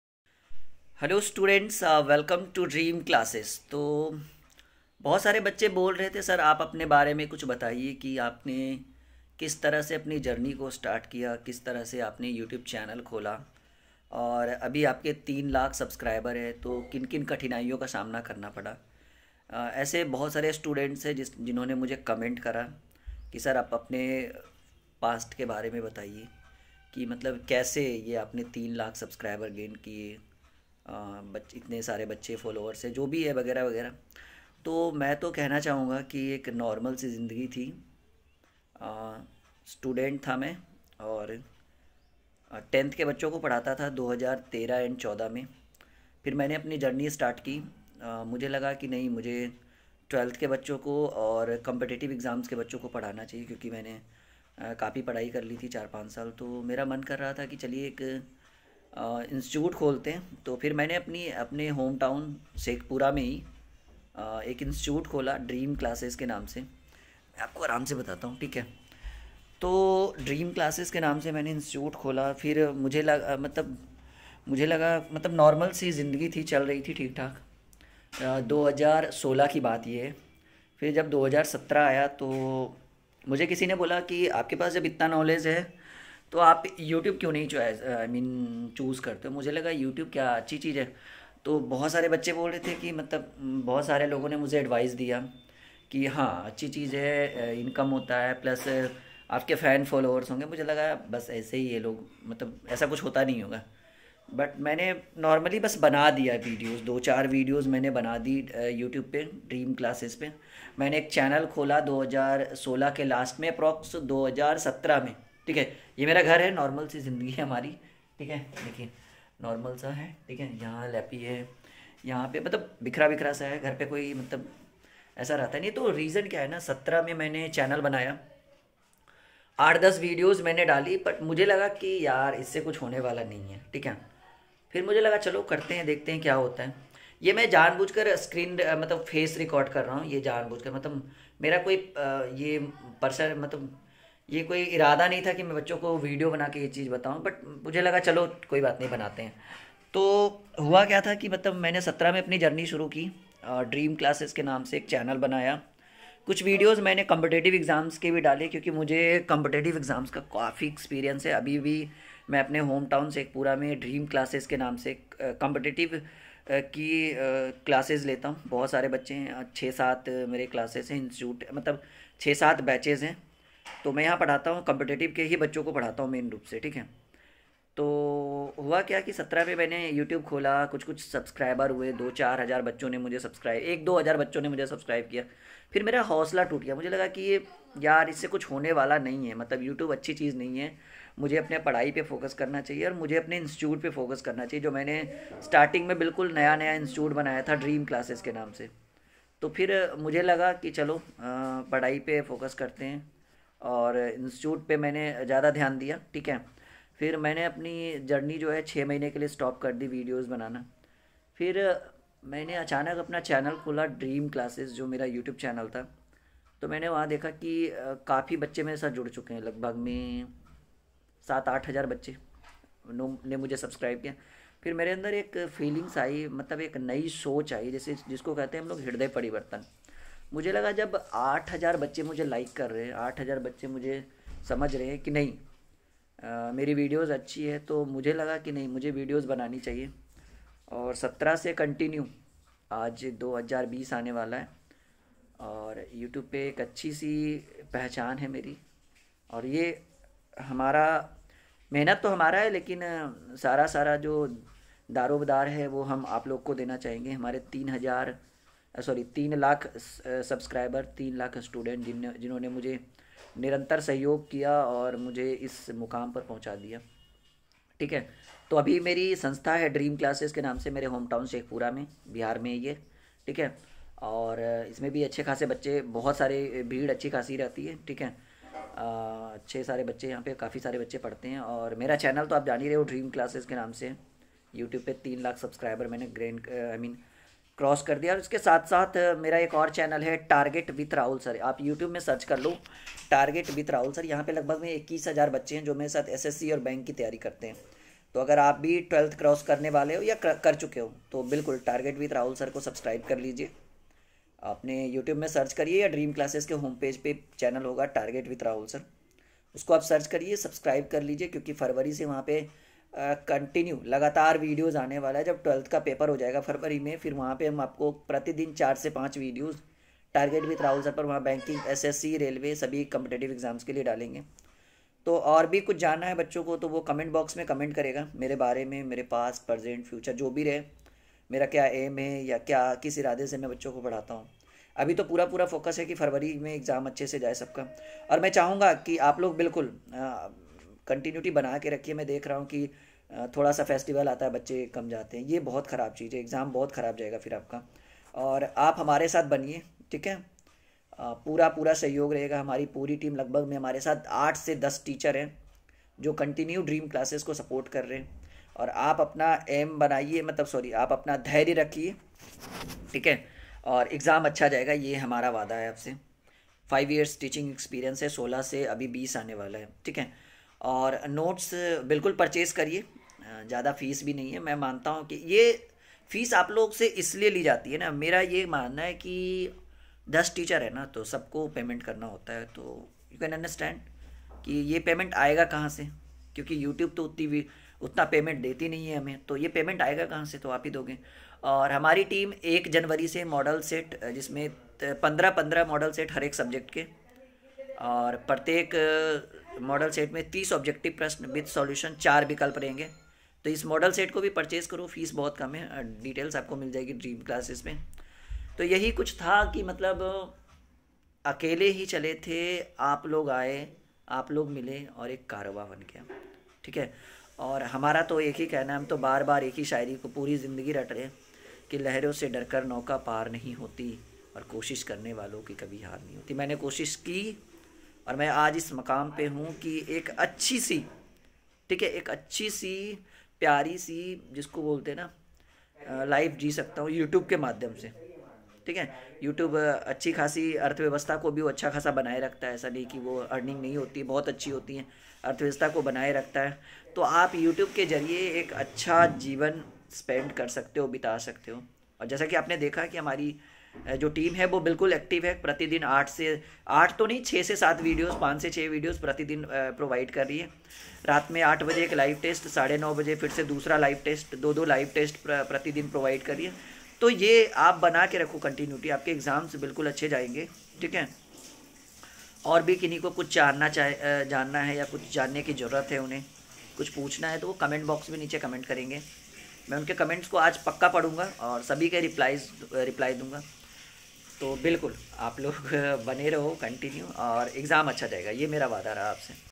DREAM CLASSES | जानिए राहुल सर की पूरी कहानी | 3 लाख SUBSCRIBERS | कैसे बना मैं YOUTUBER
RAHUL SIR ने इस वीडियो में बताया है की उन्होंने किस तरह से इतनी कठिनाइयों का सामना करके अपने जीवन में इतना बड़ा मकाम कैसे हासिल किया
DREAM CLASSES | जानिए राहुल सर की पूरी कहानी | 3 लाख SUBSCRIBERS | कैसे बना मैं
DREAM CLASSES RAHUL SIR MOTIVATIONAL VIDEO